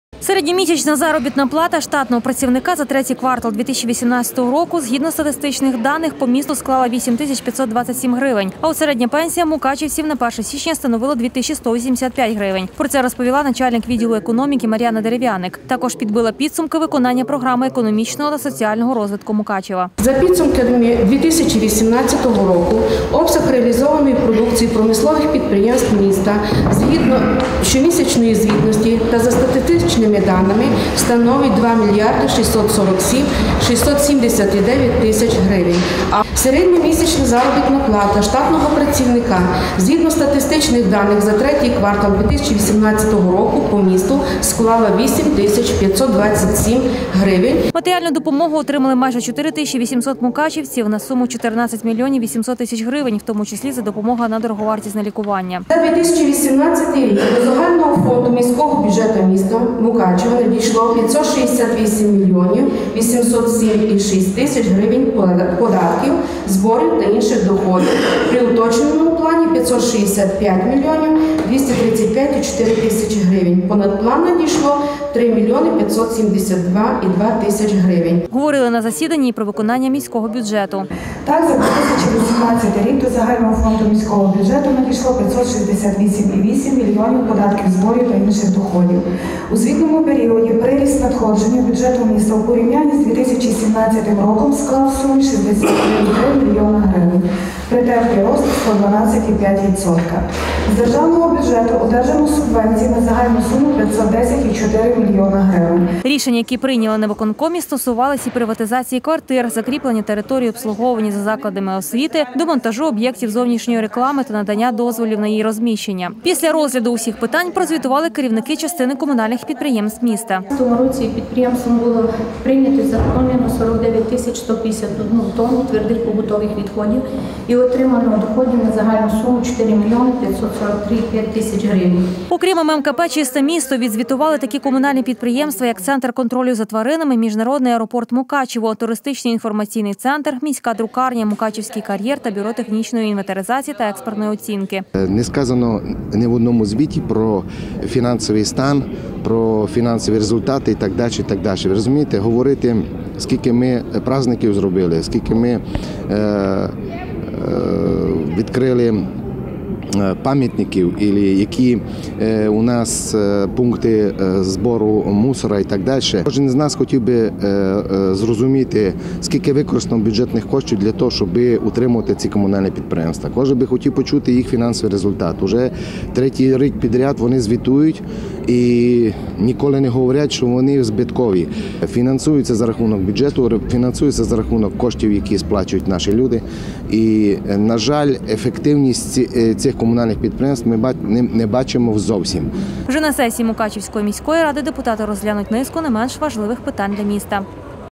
Звітували
Говорили про соціально-економічний розвиток міста та про виконання бюджету. В Мукачеві провели чергове засідання виконавчого комітету Мукачівської міської ради. Також на зібранні про свою діяльність прозвітувала низка комунальних підприємств міста.